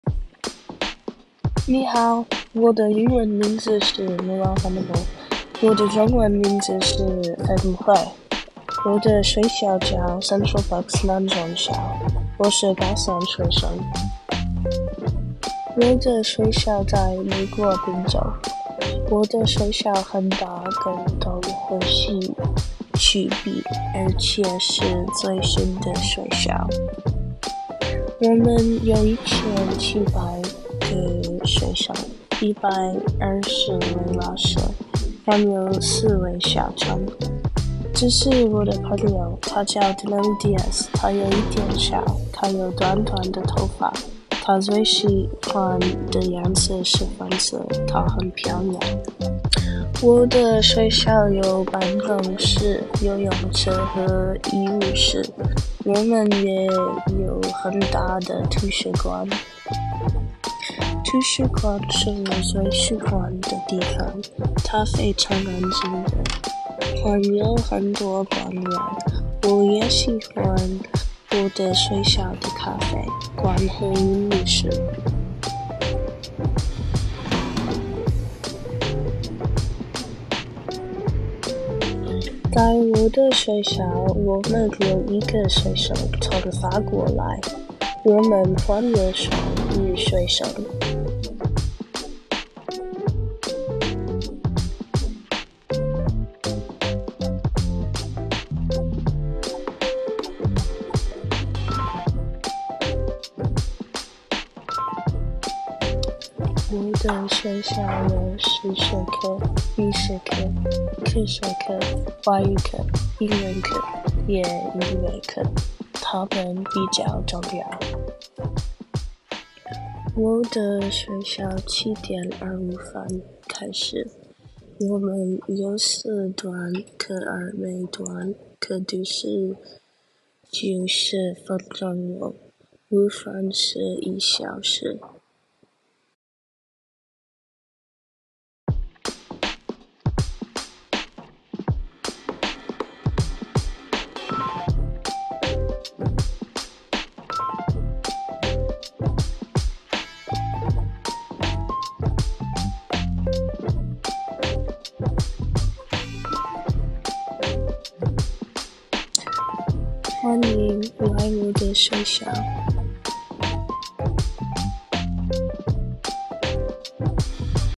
Chinese 2/3 Final Project - Introduce my school 介绍我们的学校 (CB South）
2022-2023 Final Project - people, classes, daily routines, school facilities